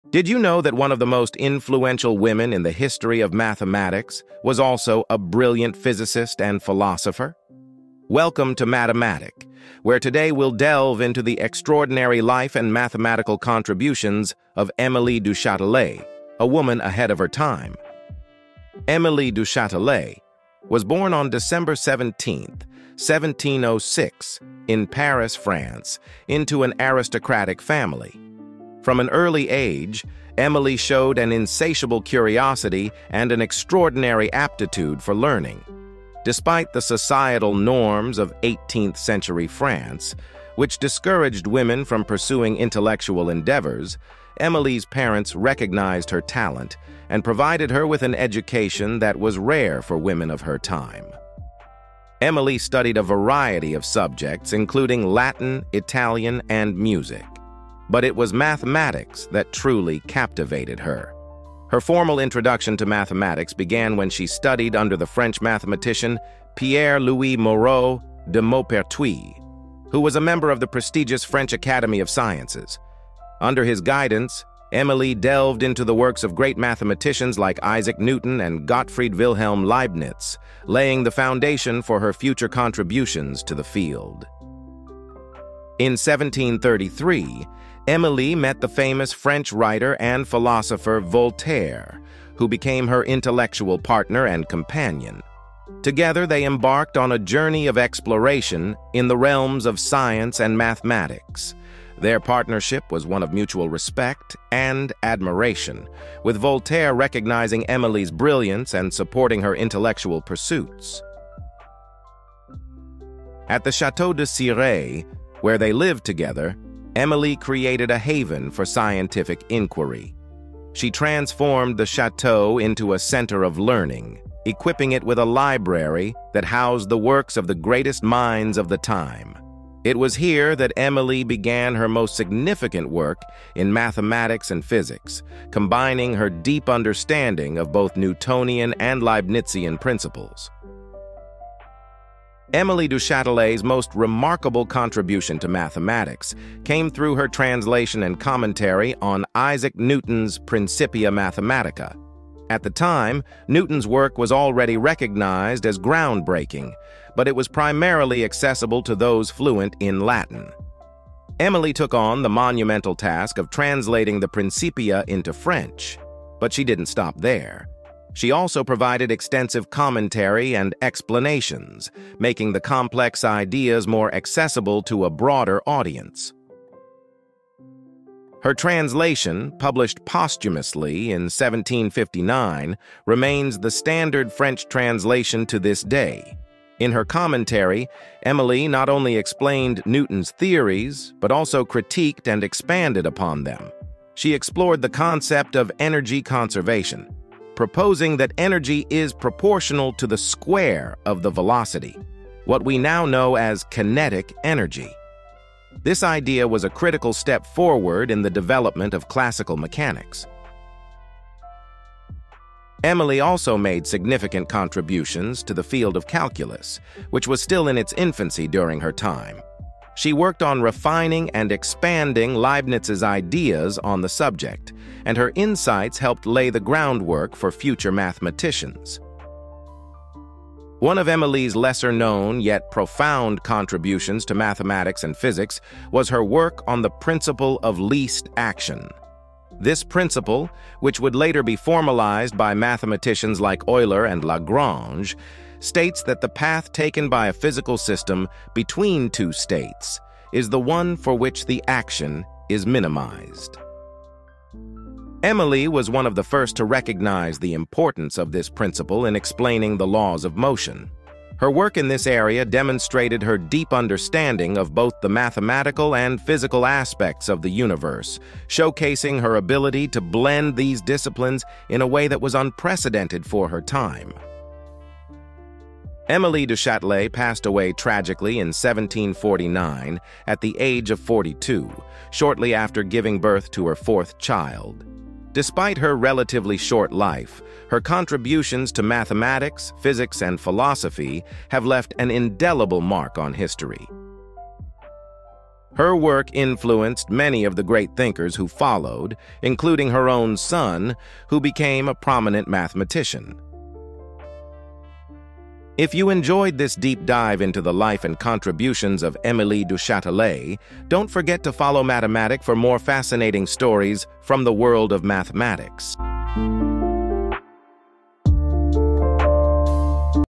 Émilie du Châtelet: The Mathematician Who Revolutionized Physics
Dive into the remarkable life and achievements of Émilie du Châtelet, a pioneering mathematician and physicist. Best known for translating and clarifying Isaac Newton’s work into French, she made groundbreaking contributions to the understanding of physics and energy conservation. Learn more about how her brilliance helped shape the scientific world, and discover the lasting impact she left on both mathematics and physics.